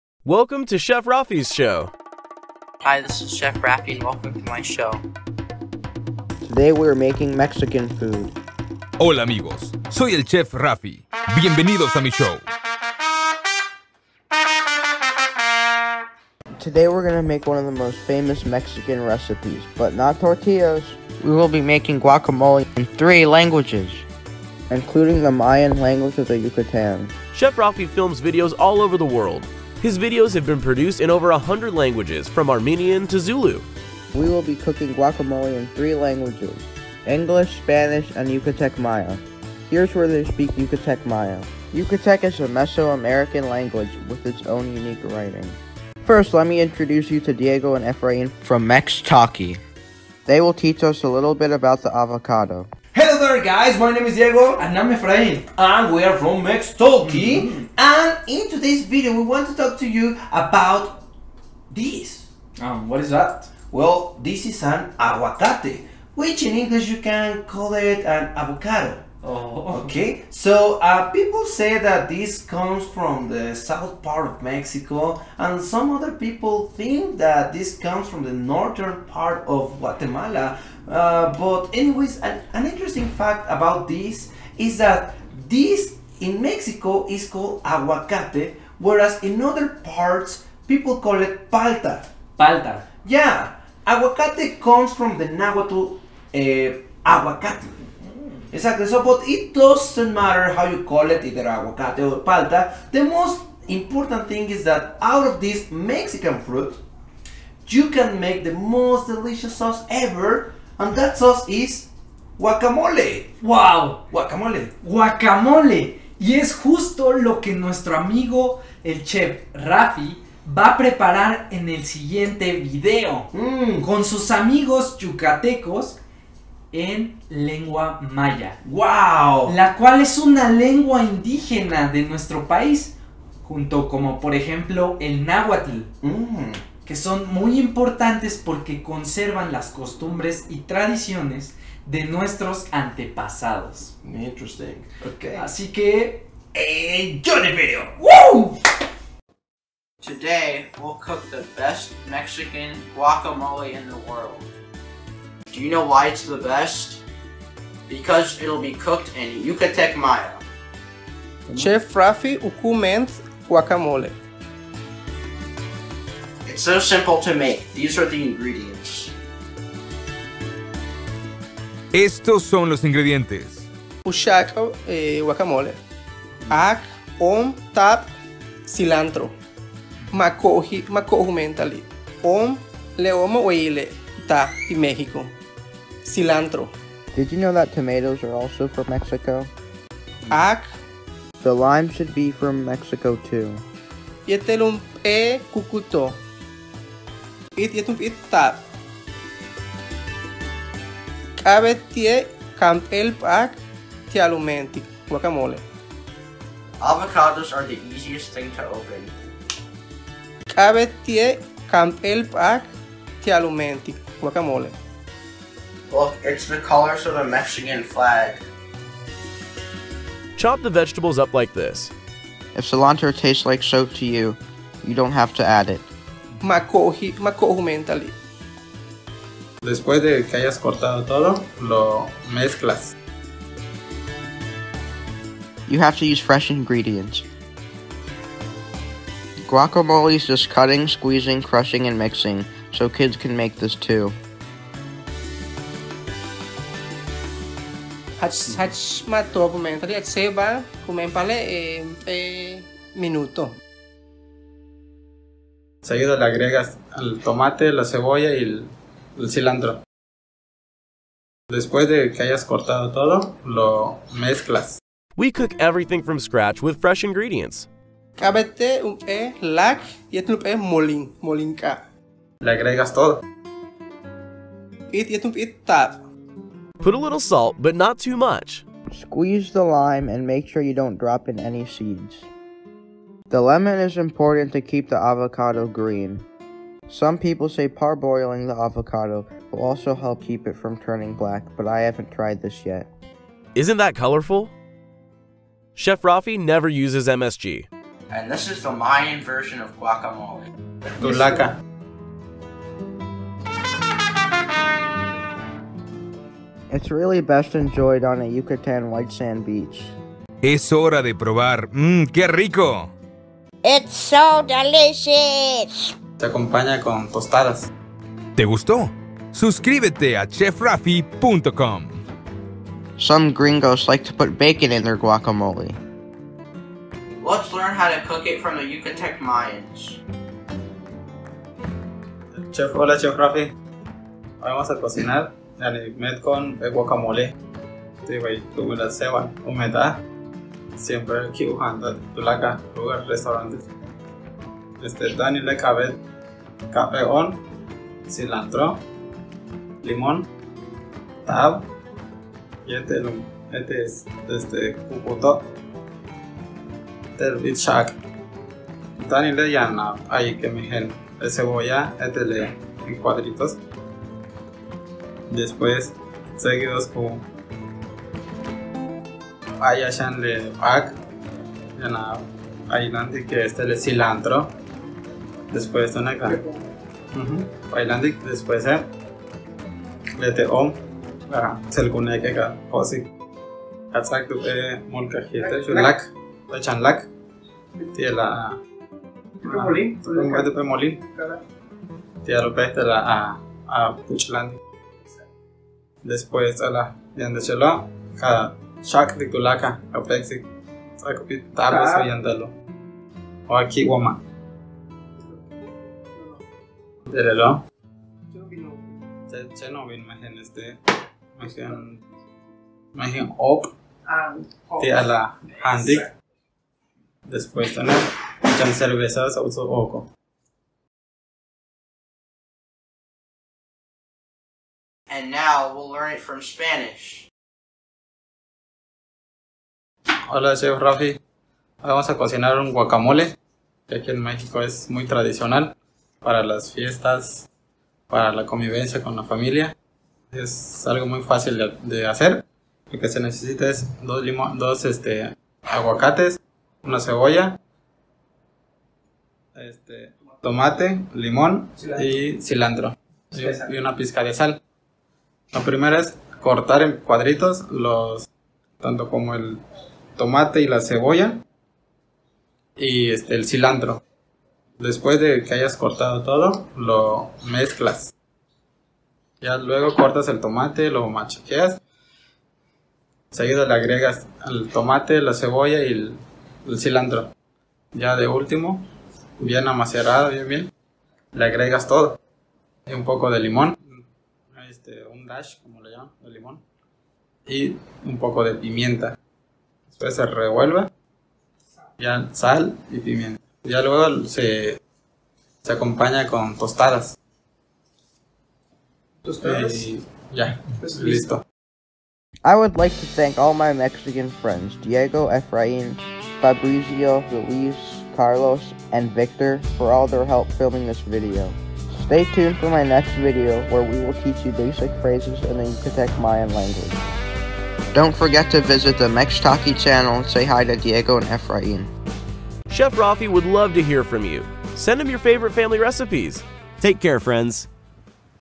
Making Mayan Guacamole in Yucatec (Trilingual Collaboration with @Mextalki DE)
Today is a very special episode for us as we are going to collaborate with the most awesome Spanish language learning channel (@Mextalki DE) and then will go to the beautiful Yucatán to show you how to make one of the simplest, but most delicious dips from Mexico: guacamole. And we will show you how to make it in English, Spanish and Yucatec Maya!

As an added bonus you’ll also hear some of Rafi’s trumpet playing. As you may know, Rafi is in three bands and loves his trumpet.

We are indebted to our amazing Mexican friends who helped make this video possible. Special thanks to Diego and Efraín from the @Mextalki channel, Luis (Yucatec video), Carlos (Yucatec audio), Fabrizio, and Victor.

Please don’t forget to visit Mextalki DE here. Diego and Efraín will definitely entertain you with their amazing videos and fantastic acting skills

Please also remember to always show respect for the indigenous languages of Mexico. They reflect the history and cultural hegemony of our continent better than anything else.